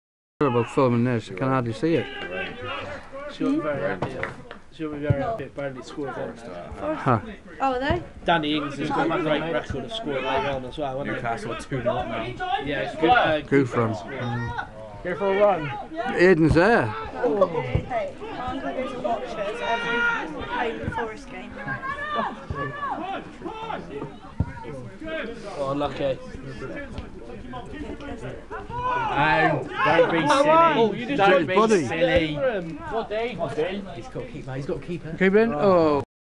Duchy Premier Division Game St Stephen 3 - 0 St Columb Major
ALL THE ACTION FROM THIS LEAGUE GAME PLAYED ON 23rd of November 2013,